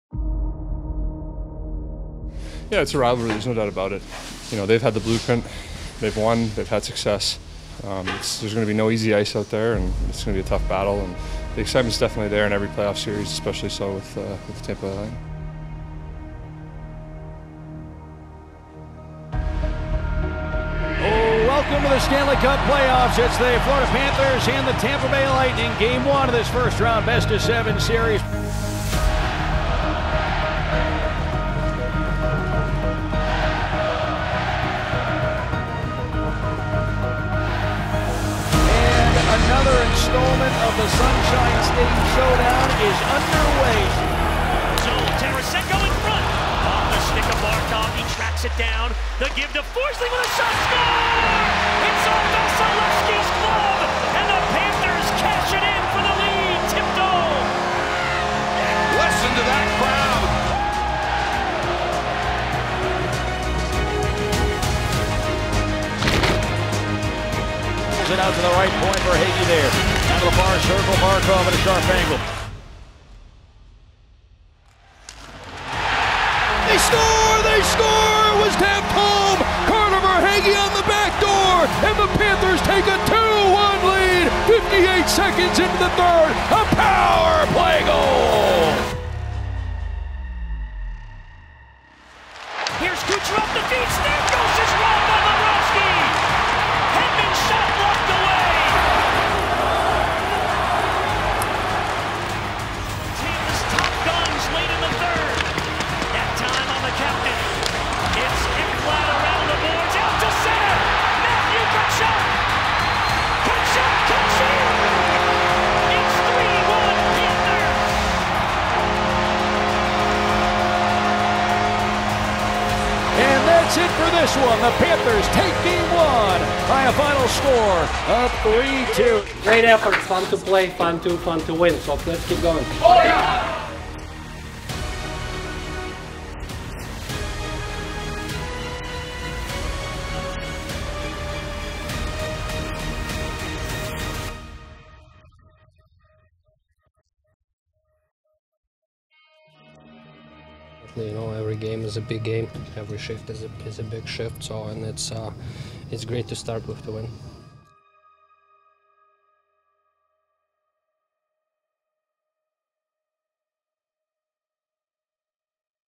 MINI-MOVIE: Cats Take Game 1 vs Lightning!
Relive Game 1 at Amerant Bank Arena as Reinhart, Verhaeghe and Tkachuk score and Bobrovsky tallies 17 saves in the 3-2 W.